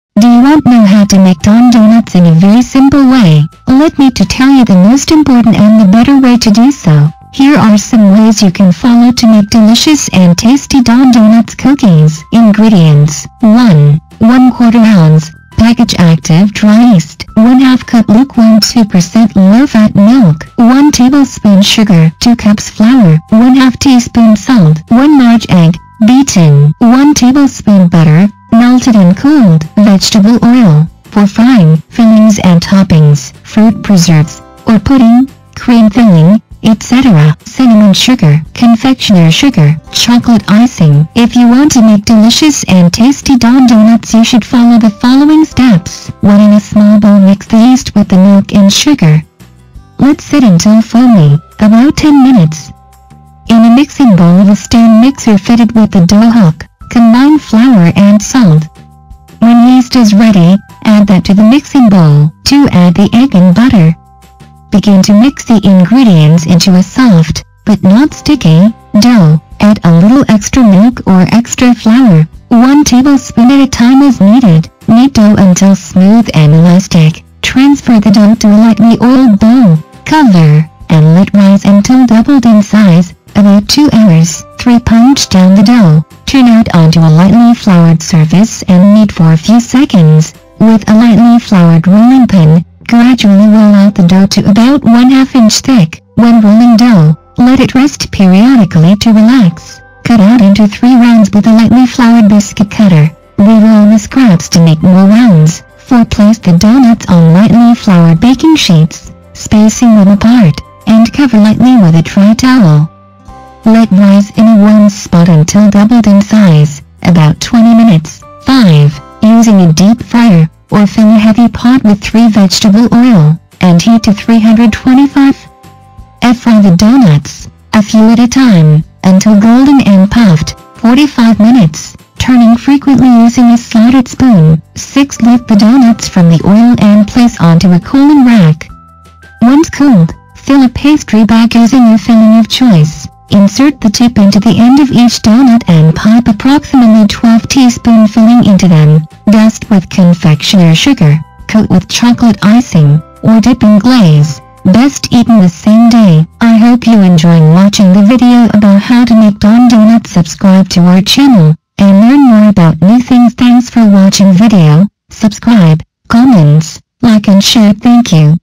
how to make don donuts-how to make don donuts at home
how to make don donuts
do you want know how to make don donuts in a very simple way?let me to tell you the most important and the better way to do so.here are some ways you can follow to make delicious and tasty don donuts cookies.

INGREDIENTS
1 (1/4 ounce) package active dry yeast
1/2 cup lukewarm 2% low-fat milk
1 tablespoon sugar
2 cups flour
1/2 teaspoon salt
1 large egg, beaten
1 tablespoon butter, melted and cooled

Fillings and Toppings

fruit preserves (or pudding, cream filling, etc)
cinnamon sugar
confectioners' sugar
chocolate icing

if you want to make delicious and tasty don donuts you should follow the following steps.

1:In a small bowl mix the yeast with the milk and sugar. Let sit until foamy, about 10 minutes. In a mixing bowl of a stand mixer fitted with the dough hook, combine flour and salt. When yeast is ready, add that to the mixing bowl.

2:Add the egg and butter. Begin to mix the ingredients into a soft, but not sticky, dough. (Add a little extra milk or extra flour, 1 tablespoon at a time as needed). Knead dough until smooth and elastic. Transfer the dough to a lightly oiled bowl, cover, and let rise until doubled in size, about 2 hours.

3:Punch down the dough. Turn out onto a lightly floured surface and knead for a few seconds. With a lightly floured rolling pin, gradually roll out the dough to about 1/2-inch thick. (When rolling dough, let it rest periodically to relax). Cut out into 3" rounds with a lightly floured biscuit cutter. Re-roll the scraps to make more rounds.

4:Place the doughnuts on lightly floured baking sheets, spacing them apart, and cover lightly with a dry towel. Let rise in a warm spot until doubled in size, about 20 minutes.

5:Using a deep fryer (or fill a heavy pot with 3" vegetable oil) and heat to 325°F Fry the doughnuts, a few at a time, until golden and puffed, 4-5 minutes, turning frequently using a slotted spoon.

6:Lift the doughnuts from the oil and place onto a cooling rack. Once cooled, fill a pastry bag using your filling of choice. Insert the tip into the end of each doughnut and pipe approximately 1-2 tsp filling into them. Dust with confectioners' sugar, coat with chocolate icing, or dip in glaze. Best eaten the same day.

best way to make don donuts must watch this video.